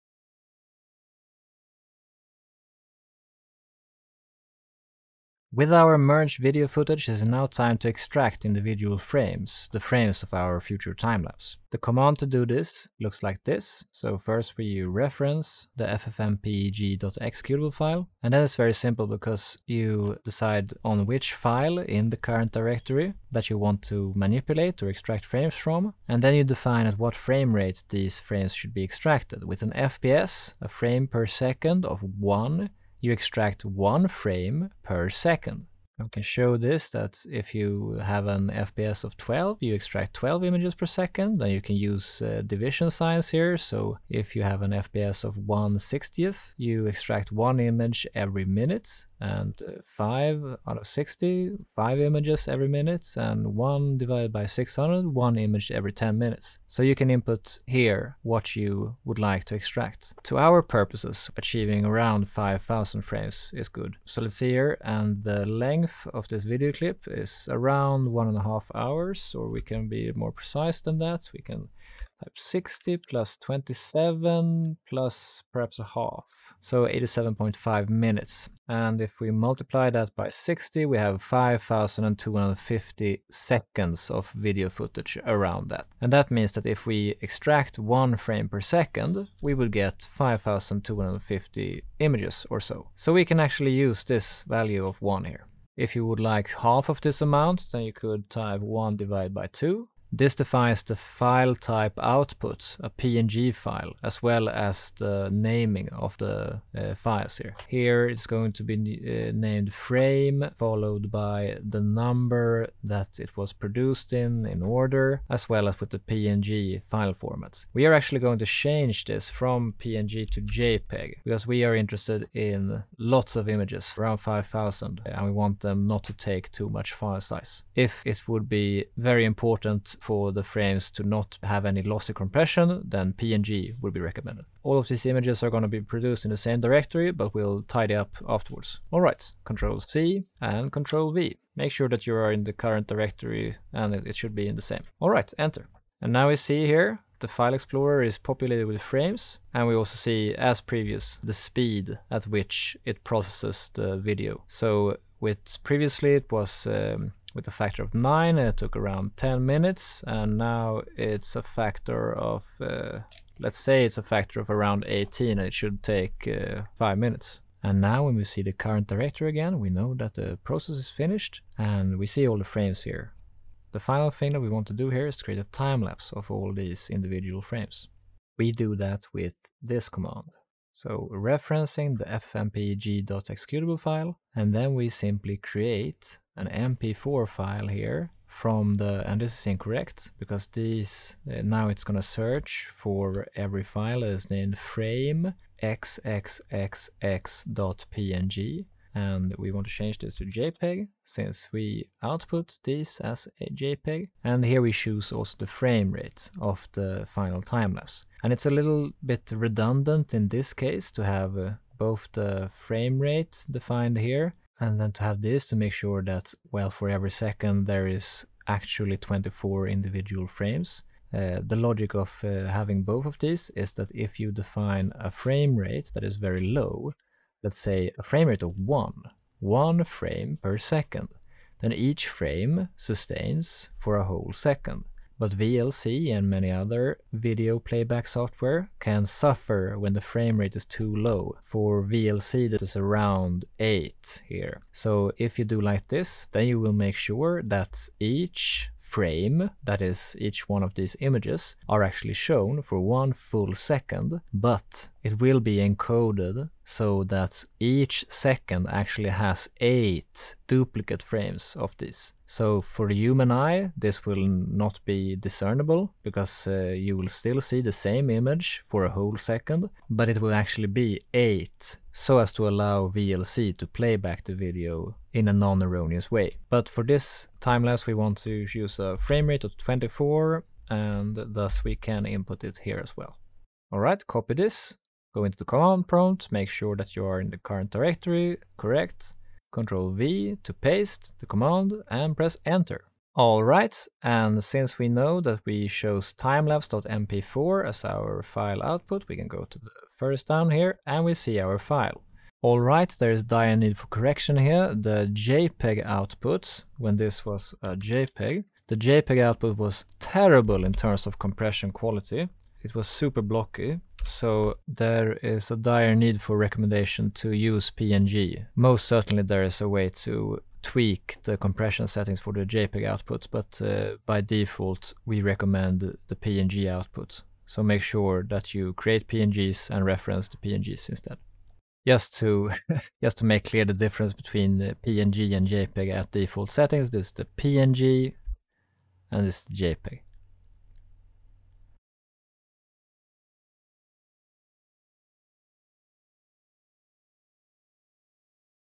5 minutes on: File management – Extract Frames and Create Timelapse Using FFmpeg [FFmpeg: Part 2]
This is a very brief 5-minute video on the procedure of using FFmpeg in Windows to extract frames from a video file and create a timelapse from the extracted frames. This builds upon the foundation of a preceding tutorial, where multiple video clips were merged into one video file. This tutorial shows only the second part of the full workflow, and it is recommended to have viewed the first part.

Timestamps:
0:05 Extracting individual image frames from video file
2:53 Creating a timelapse from the extracted frames
5:02 Correction of recommended workflow; PNG quality compared to default JPG compression
5:41 Sampled timelapse

Command Prompt commands:

For all of the below, make absolutely sure that the current directory is correct. If unsure, please watch the previous tutorial (linked above).

To extract frames from a video file using FFmpeg, input:
[DIRECTORY TO ffmpeg.exe] -i [NAME OF FILE IN CURRENT DIRECTORY TO EXTRACT FROM] -vf fps=[FRAMERATE FOR FRAME EXTRACTION] [OUTPUT PREFIX]%04d.[OUTPUT FILE TYPE]

Example of above:
C:\Users\aifoskela\Desktop\ffmpeg-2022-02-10-git-b6bb6b9f22-essentials_build\bin\ffmpeg.exe -i Merged.mp4 -vf fps=1 frame%04d.png

Examples of fps=X values to define as extraction framerate:
fps=1
// one image every second
fps=12
// 12 images per second
fps=1/60
// one image every minute
fps=5/60
// 5 images every minute
fps=1/600
// one image every ten minutes

To create a timelapse from individually extracted frames using FFmpeg, input:
[DIRECTORY TO ffmpeg.exe] -f image2 -framerate [FRAMERATE FOR INPUT FRAMES] -i [PREFIX TO SEARCH FOR]%04d.[FILETYPE TO SEARCH FOR] -r [OUTPUT FRAMERATE] [FILENAME OF OUTPUT FILE]

Example of above:
C:\Users\aifoskela\Desktop\ffmpeg-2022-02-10-git-b6bb6b9f22-essentials_build\bin\ffmpeg.exe -f image2 -framerate 24 -i frame%04d.png -r 24 Timelapse.mp4

N.B.! If you want JPEG output for the extracted frames, but with higher quality than default, you may add "-qscale:v 2" immediately after the input file, e.g.:
C:\Users\aifoskela\Desktop\ffmpeg-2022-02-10-git-b6bb6b9f22-essentials_build\bin\ffmpeg.exe -i Merged.mp4 -qscale:v 2 -vf fps=1 frame%04d.jpg
The default quality scale range of JPEG output in FFmpeg is from 2 (best quality) to 31 (worst),